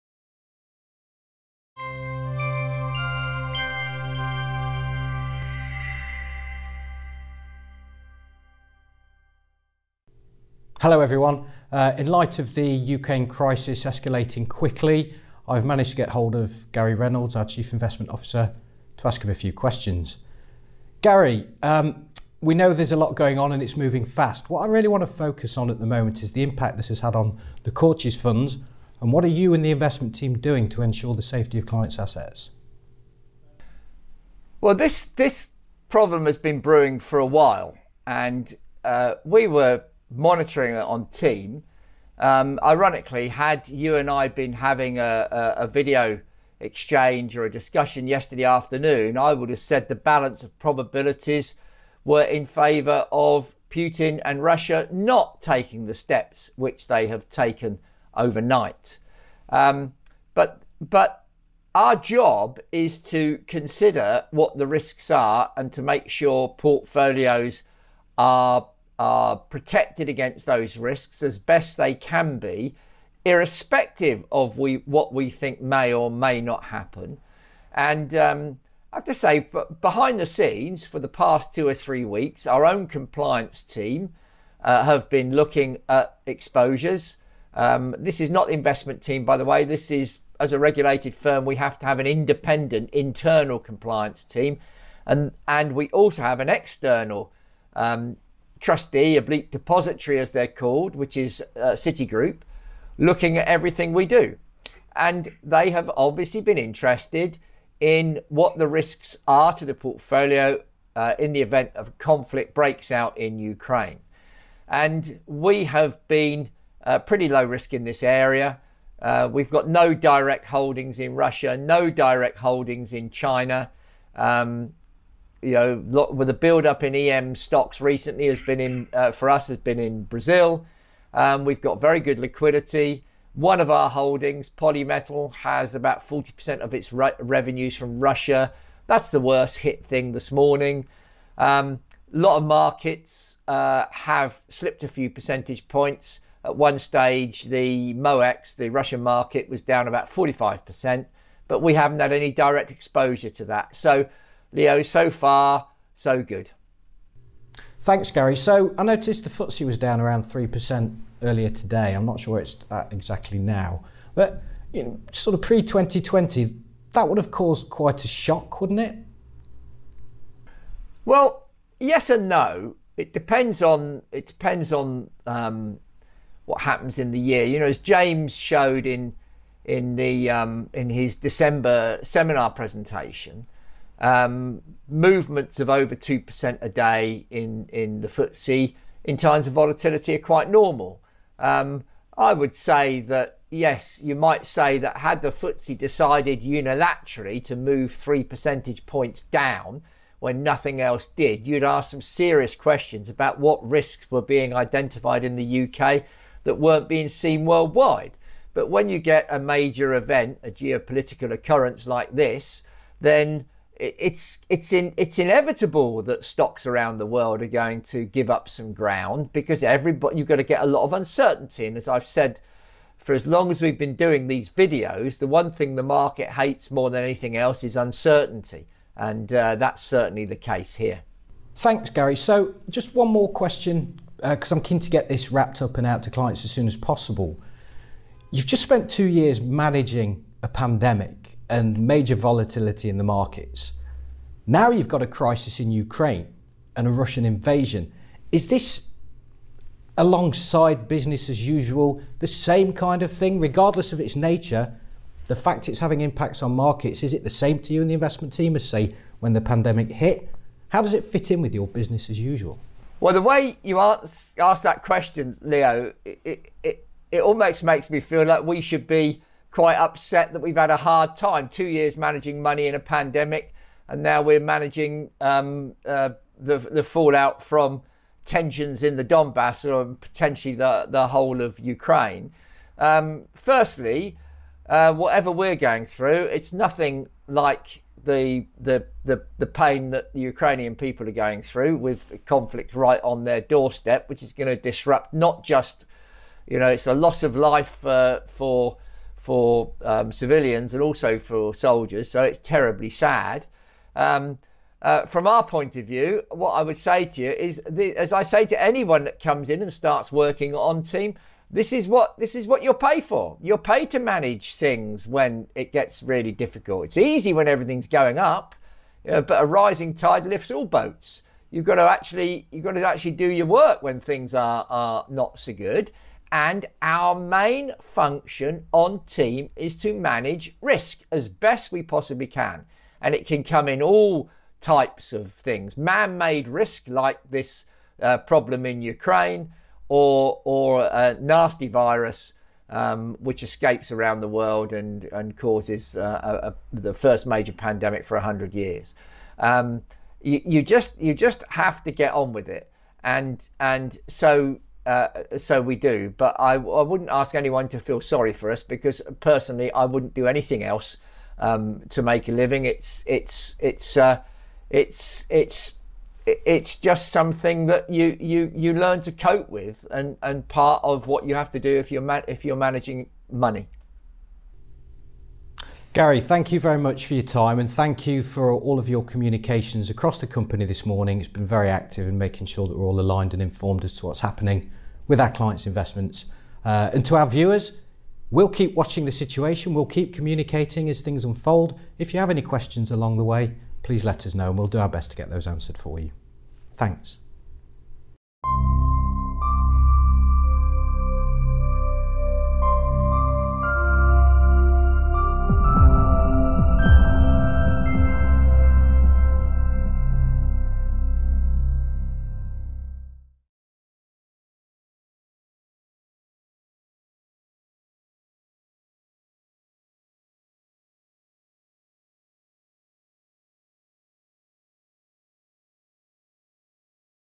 Ukraine Crisis and impact on Courtiers Funds
Gary explains the impact of the Ukraine Crisis on fund performance and investment strategy, while Leo asks about the pressures that come with managing one cause of volatility to the next.